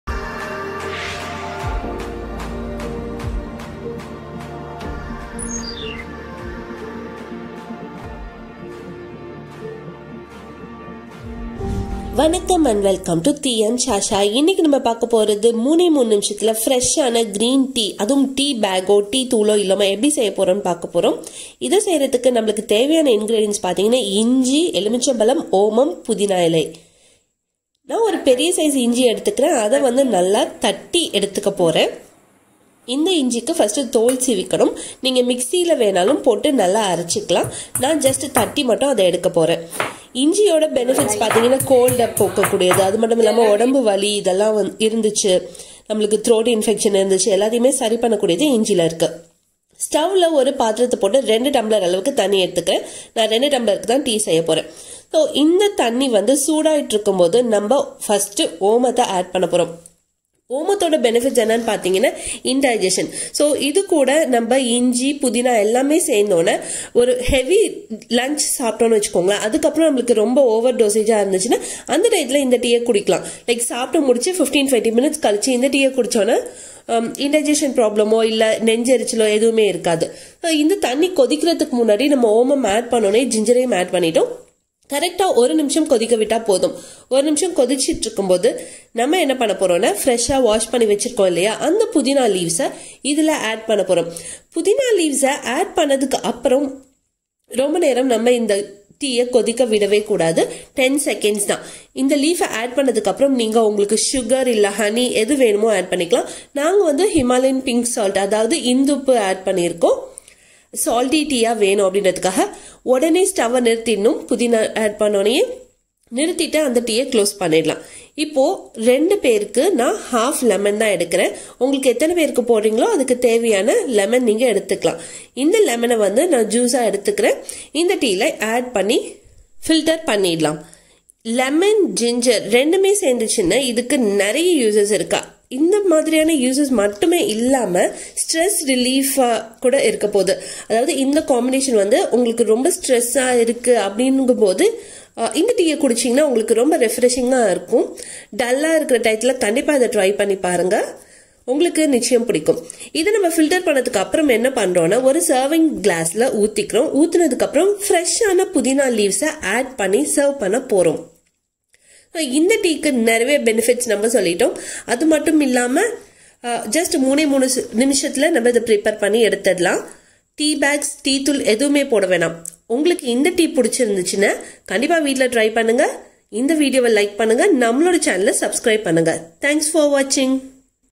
Green Tea Without Tea Bag| Green tea after Heavy lunch |Quick fresh Green Tea
3minutes tea making
Prep Time 1 minute
Cook time 1 minute
Serve time 1 minute

step 1  Remove the skin of Ginger and crush it
step 2 Add water and heat in a bowl
step 3 Add Ajwain/omam and Ginger. Let it boil for a minute
step 4 Add pudina /mint leaves and stop the stove. Add Honey or Salt
step 5 Add lemon juice and Filter
easy step 6 Add fresh mint leaves and serve hot